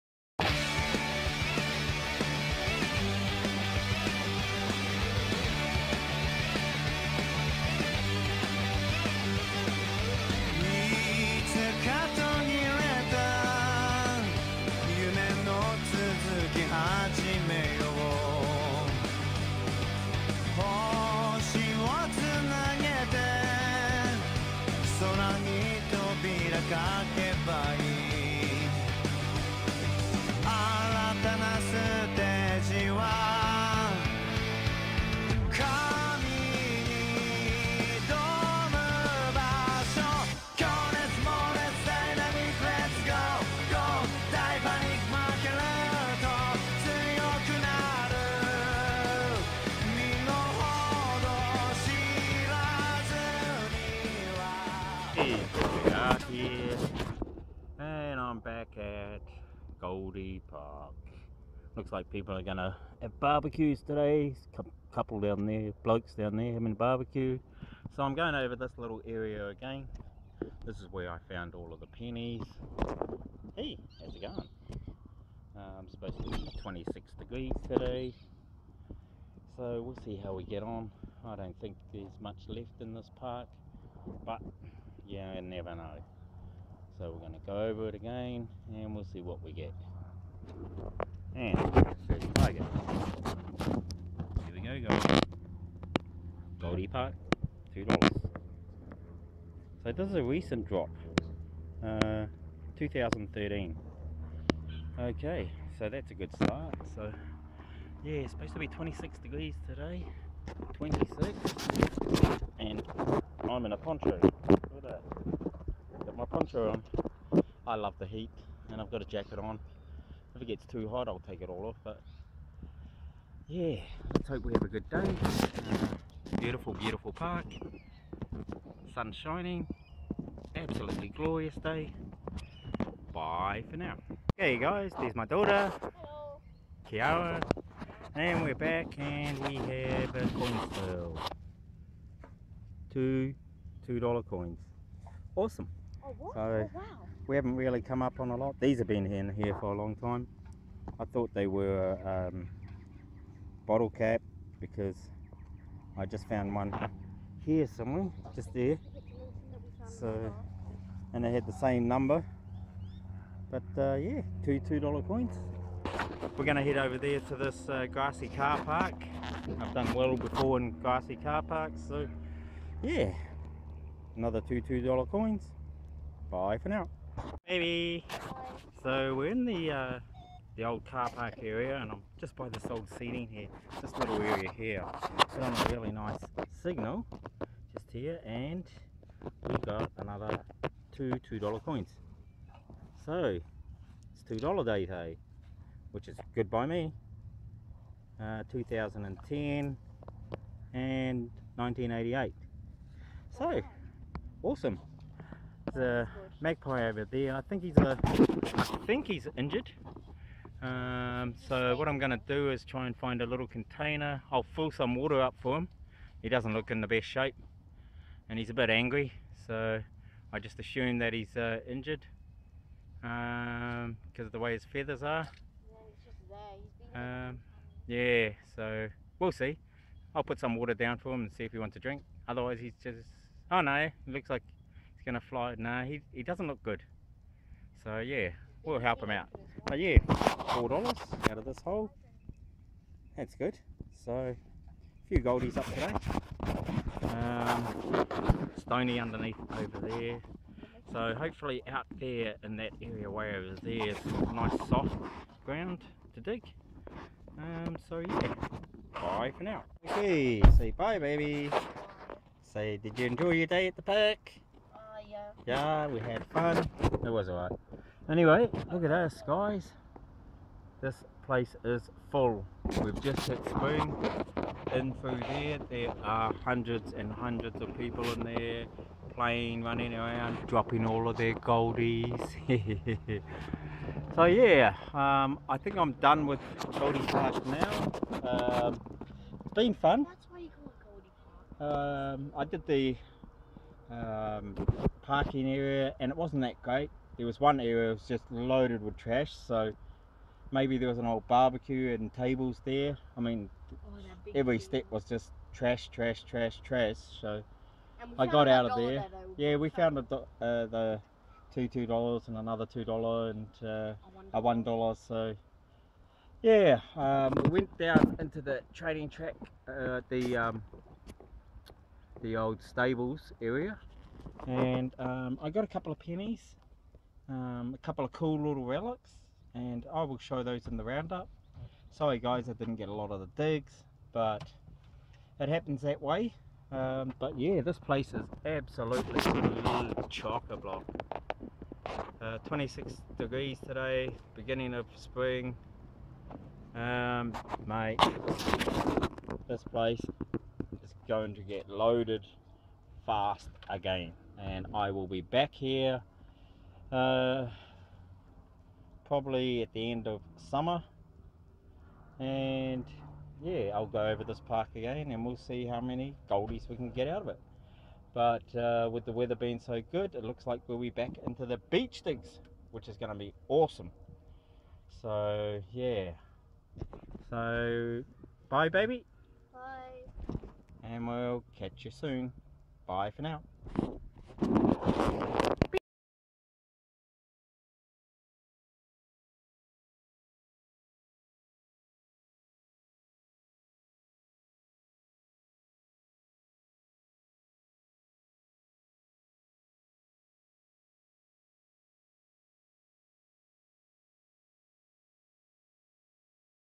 Short hunt - Metal Detecting Goldie Park
Had a good day with my Daughter but it was Hot and I never got to film as much as I would have liked too.
Sorry about the round up which stuffed up.